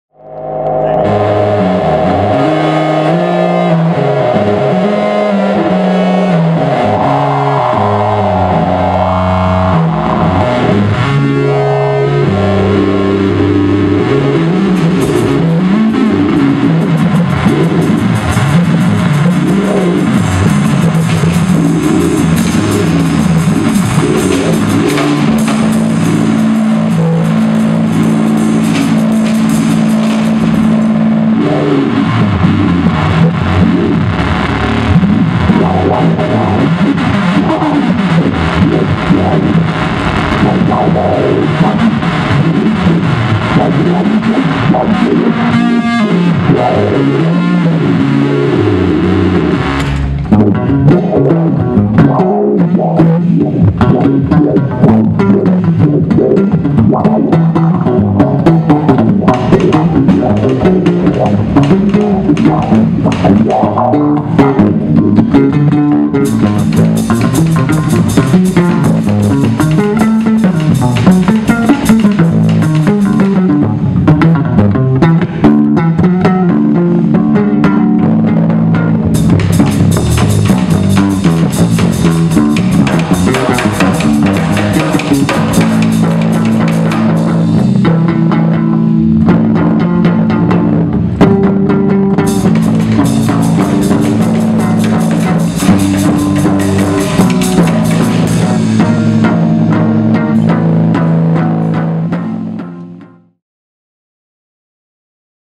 Melvin Gibbs / Harriet Tubman Plays Free Jazz / WinterJazz Fest
Melvin Gibbs guitar solo Winter Jazz Fest NYC 2018